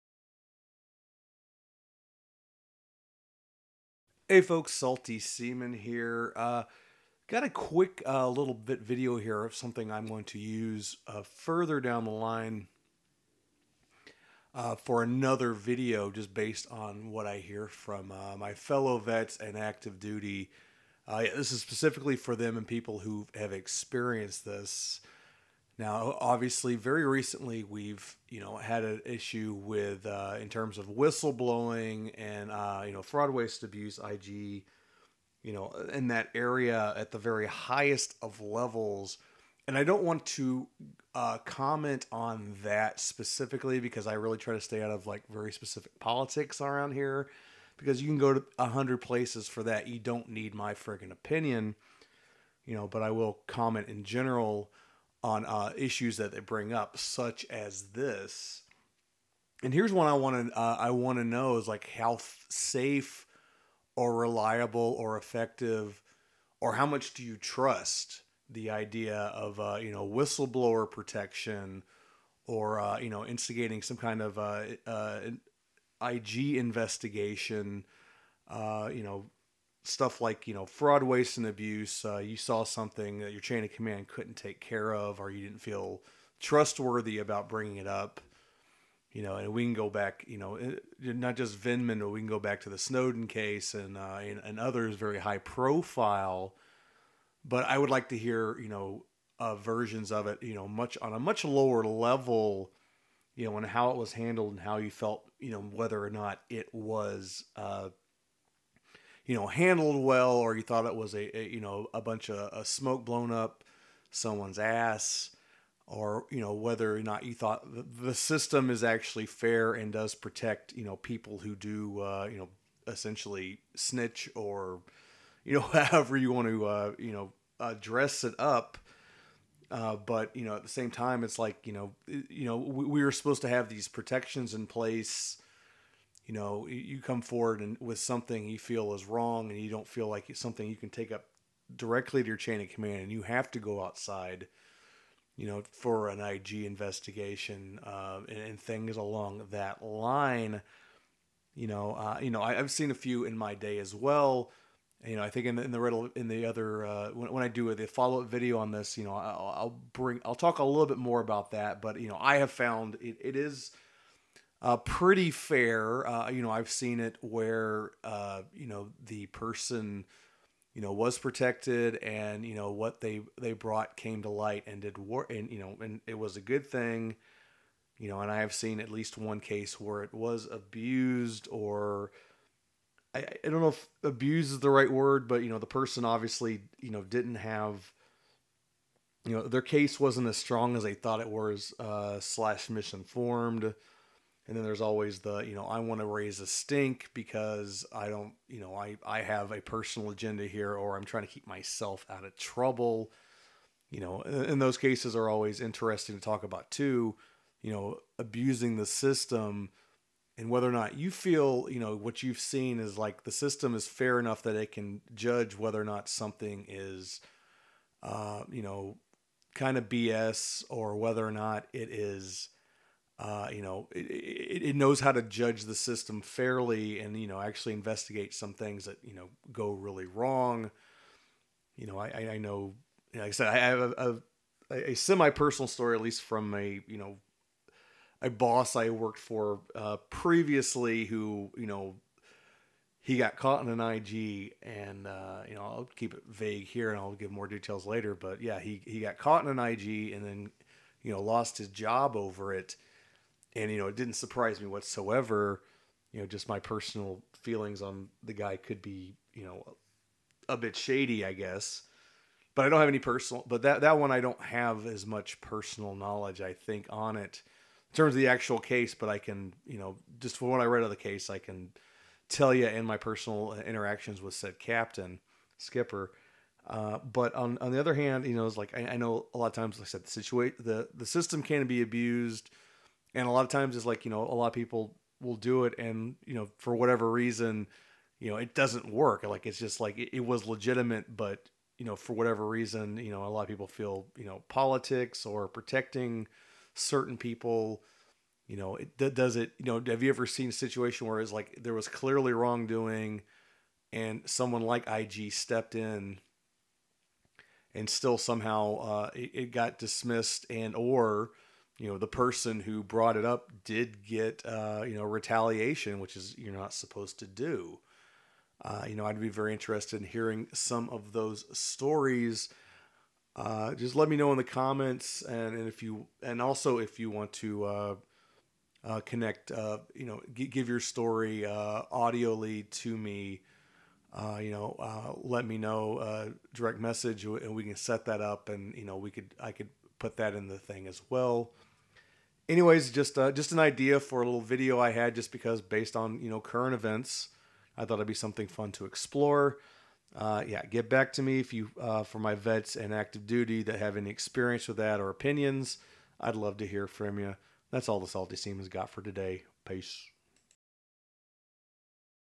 Is The Military Whistleblower System FAIR?
Wanting to hear from my vets and active duty.  Give me your stories either in comments or via email/skype for a future video.